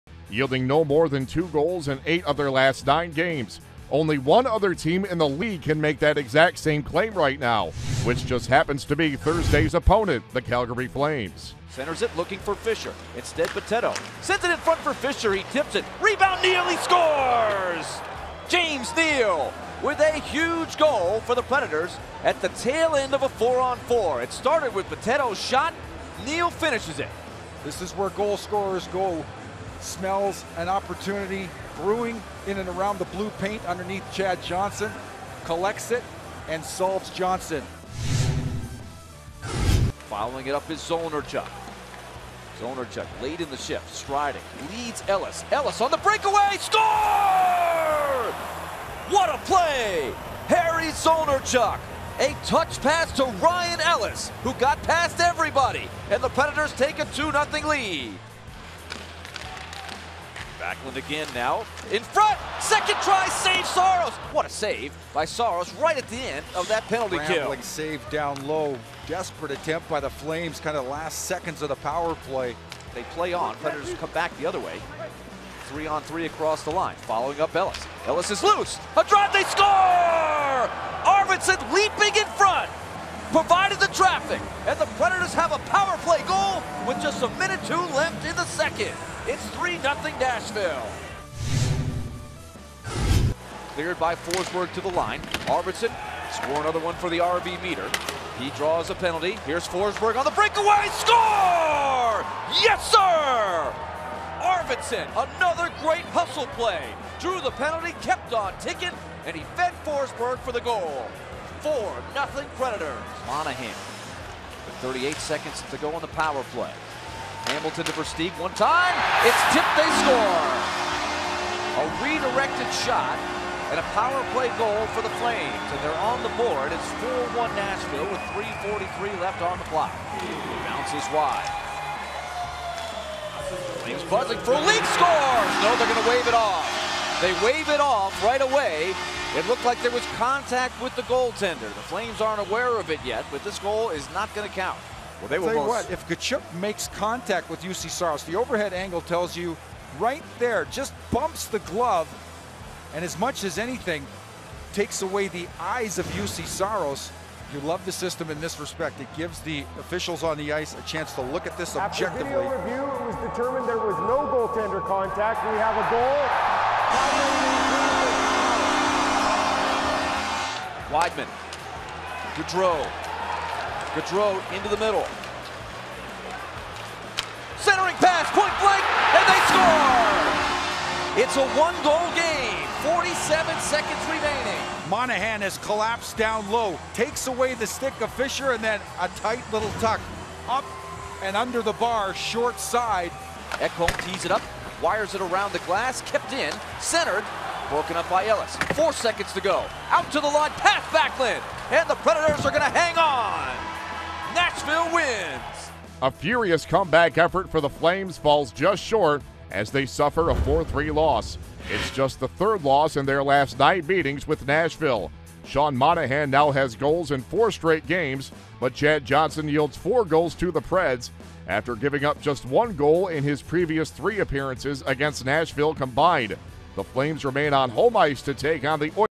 Ellis' goals, Saros' 32 saves lead Preds past Flames[Mark Schwarzer]
Juuse Saros recorded 32 saves while Ryan Ellis bagged a pair of goals in the Predators' tight 4-3 road victory over the Flames